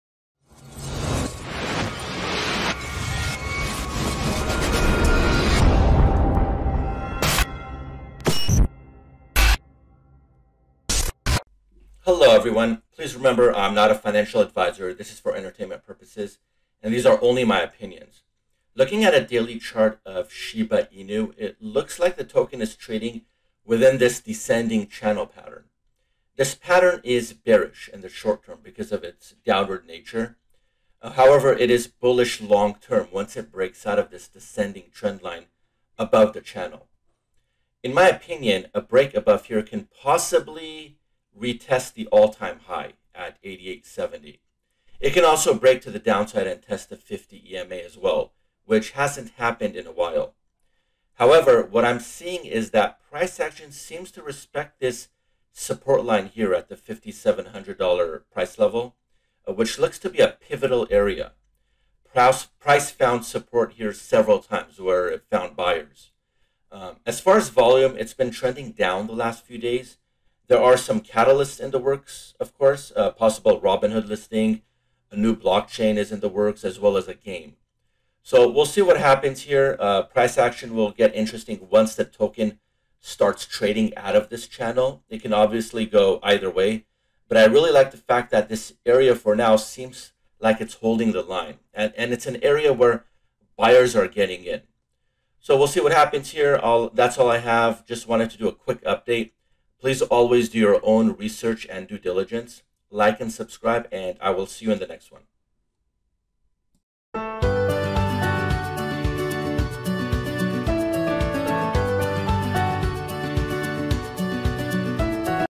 SHIBA INU UPDATE!!! LONG-TERM BULLISH DESCENDING CHANNEL!!
SHIBA INU UPDATE!!! BUYERS STEPPING IN! QUICK LOOK AT A DAILY AND FOR SHIBA INU SHOWING HOW PRICE CAN BREAK OUT OF DESCENDING CHANNEL PATTERN.

Shiba Inu coin is showing resilience as it is being bought up after it dips. Shiba Inu coin is holding support on the stock chart.